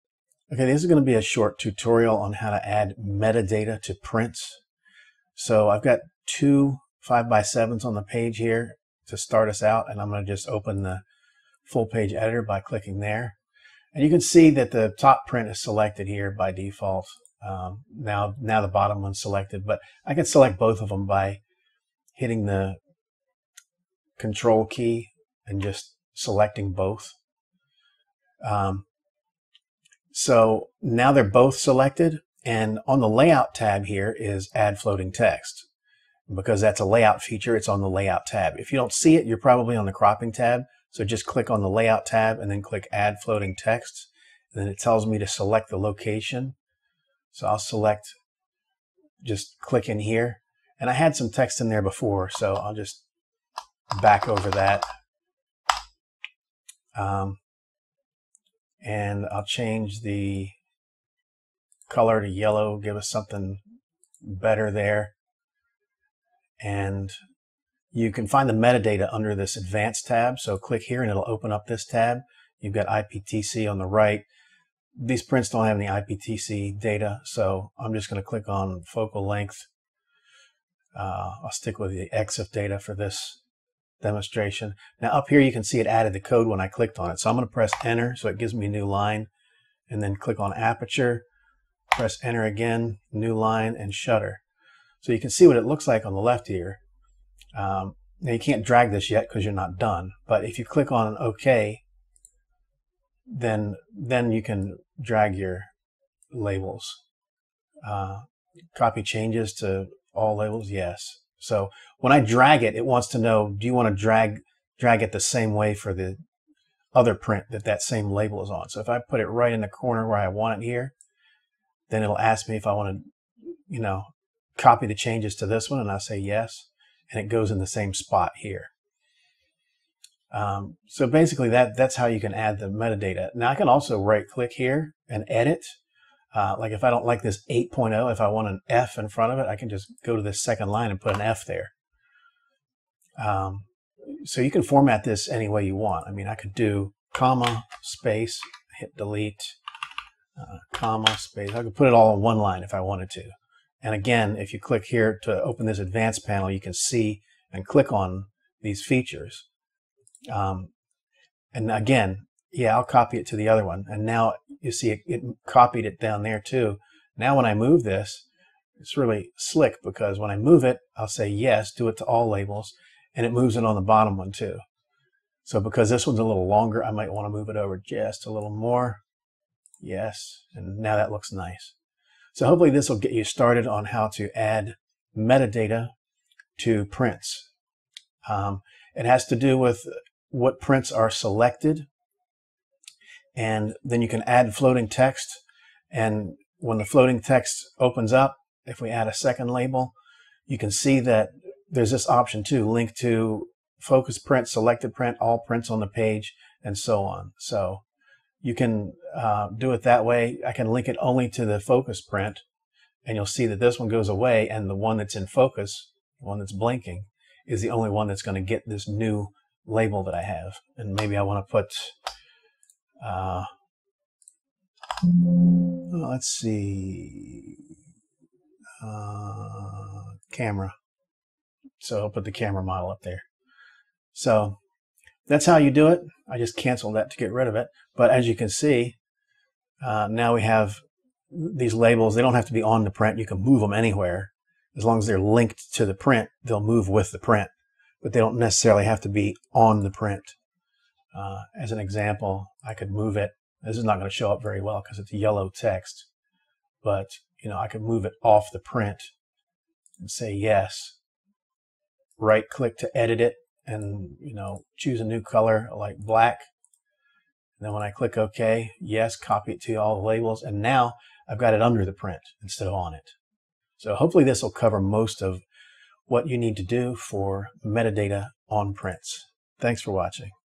Qimage Ultimate - Adding Metadata to Prints
In this video answer, we demonstrate how to add metadata on or below prints on the page.